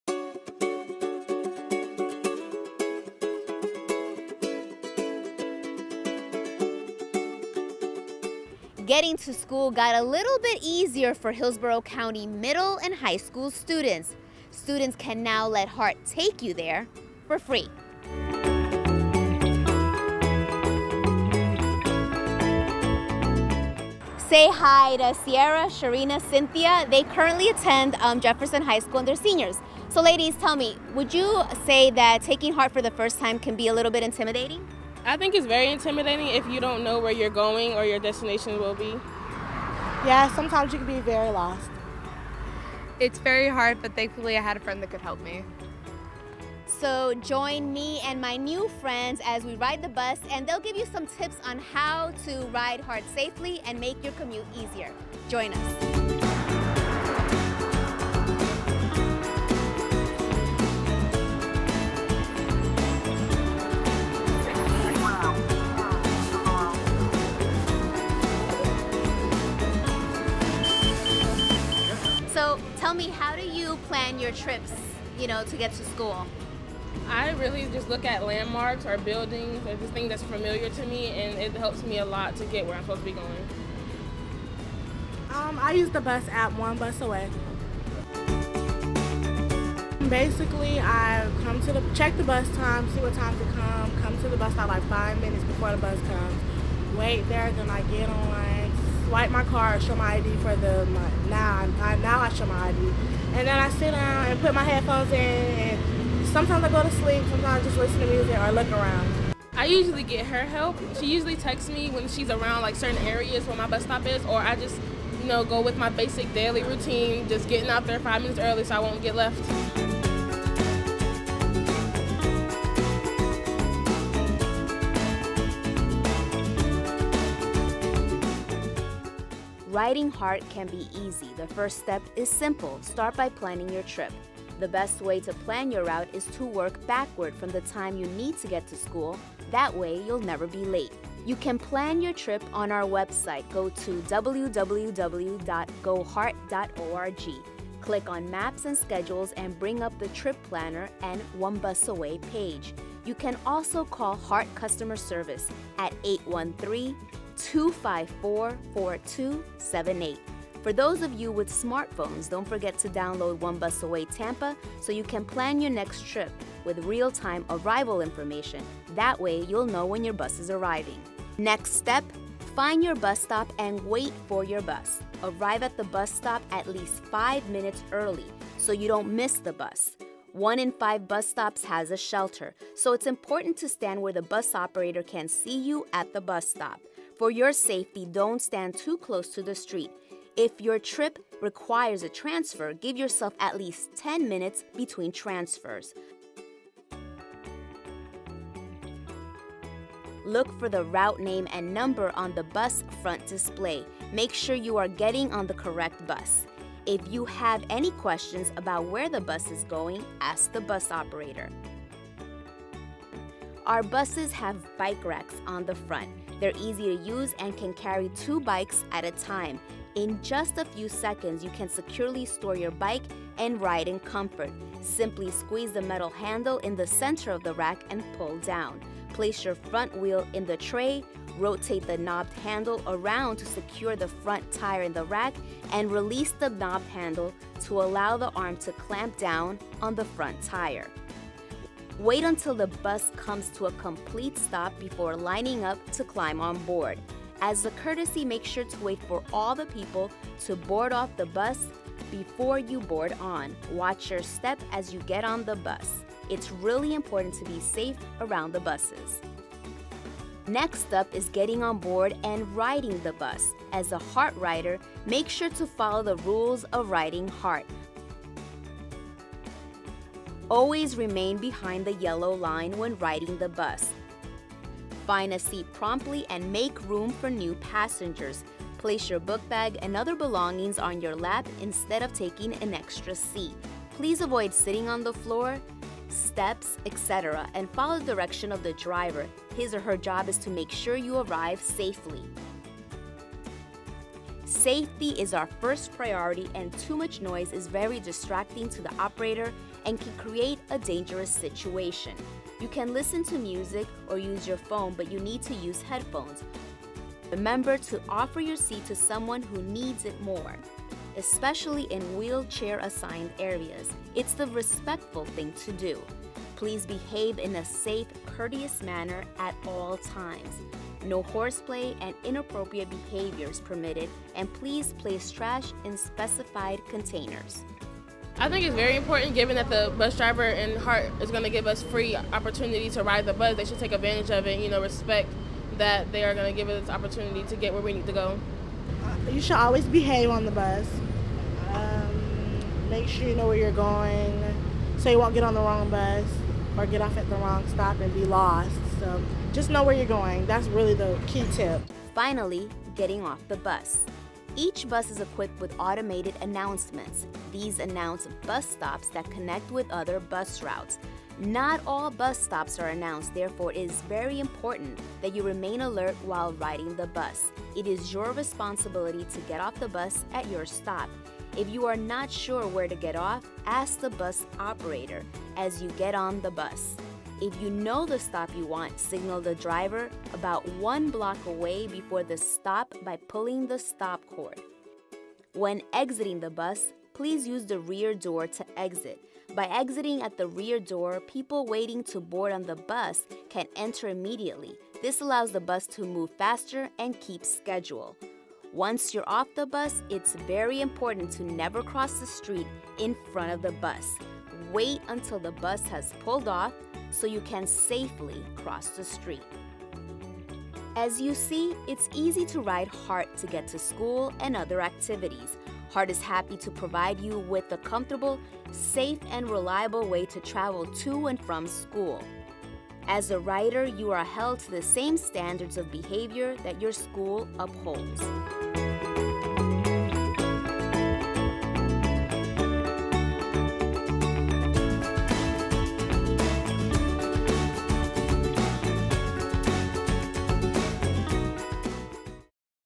How to Ride HART
HART staff (with help from Jefferson High students) shows you how easy it is to ride HART!

Video credit: City of Tampa Television